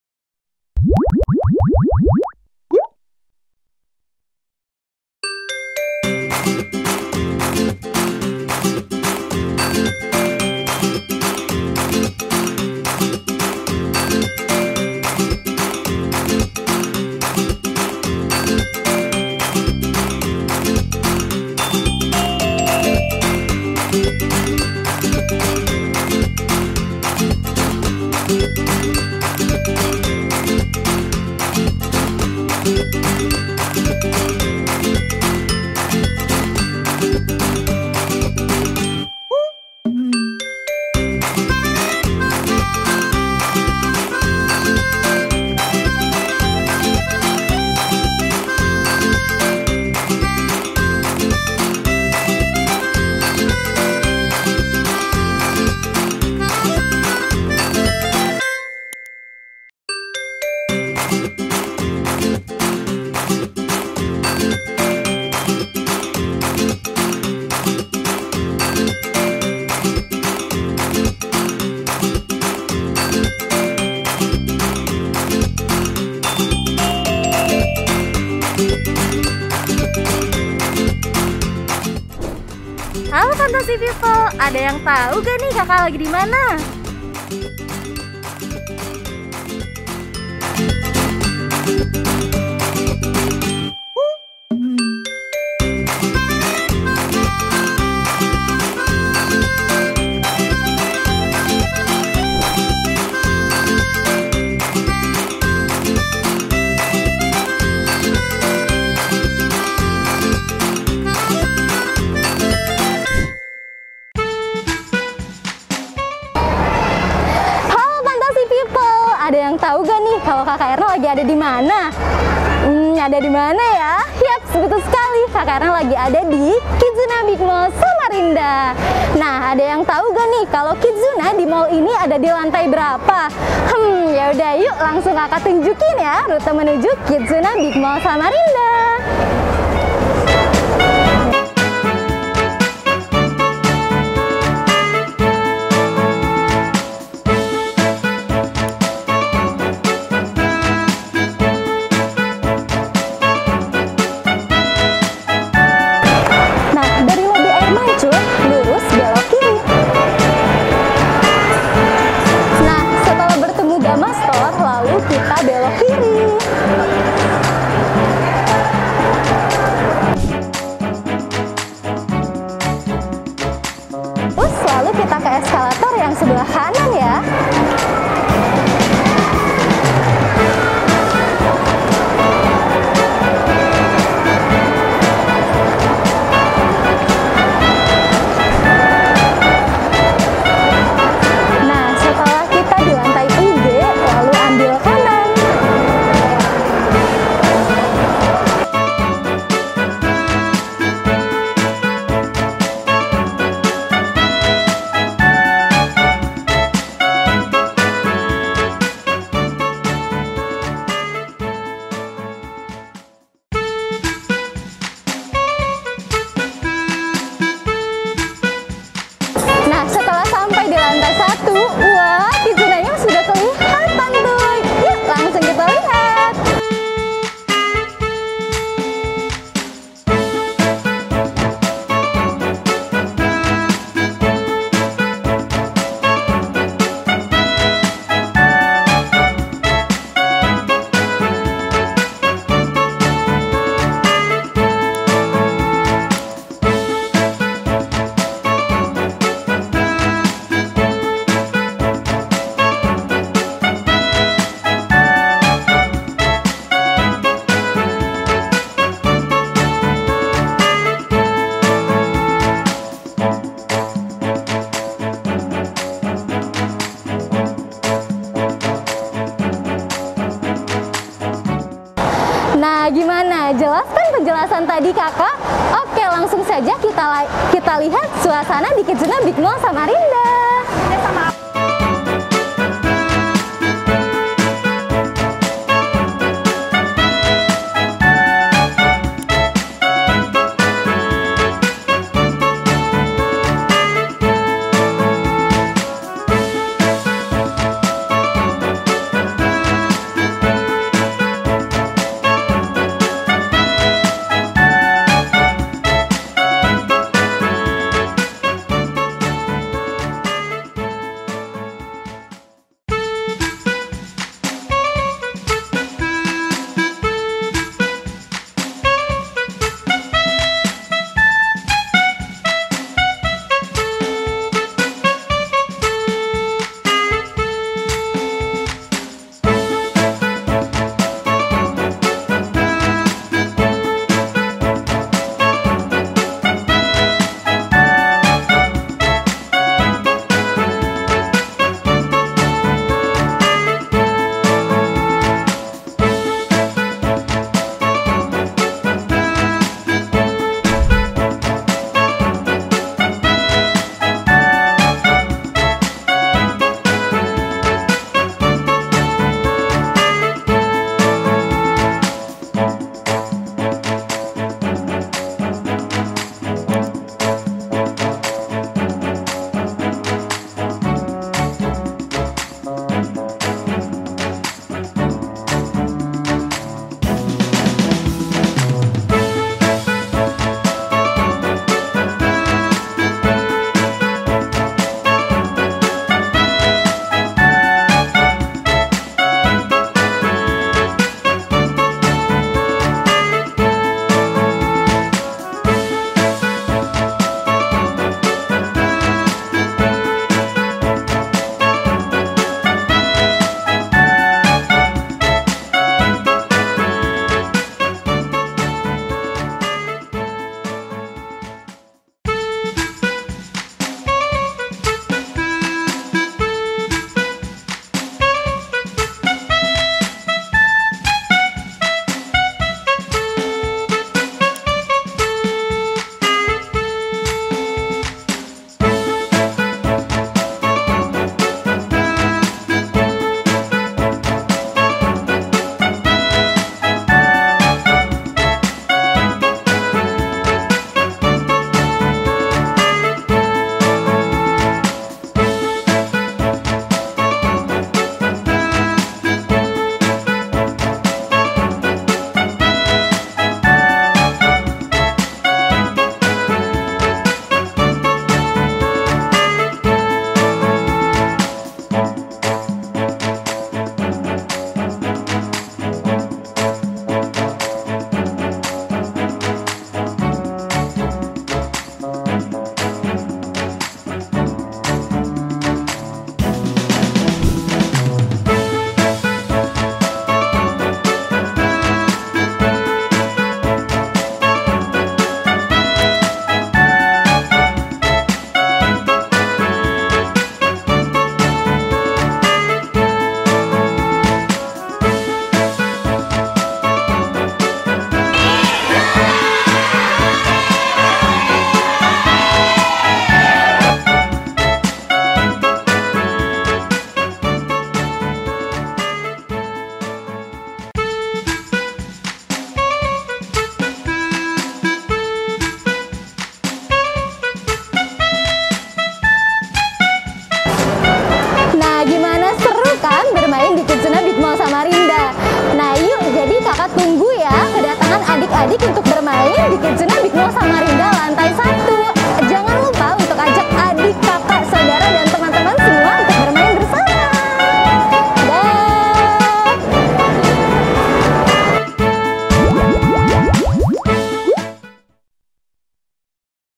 [STORE kidzooona] Bigmall Samarinda Lantai 1
Hula Fantasy People...

Bagi Fantasy People yang berada di Kalimantan Timur, tepatnya di Samarinda, tidak sulit untuk bermain di kidzooona, karena kami buka cabang di Big mall.

Nah ingin berkunjung ke store kidzooona ini, dapat langsung menuju Bigmall Lantai 1.

Seperti apa nuansa storenya ?
yuks kita intip.

Yuks kita tonton video berikut.

Facebook : kidzooona & Molly Fantasy Aeon Fantasy Indonesia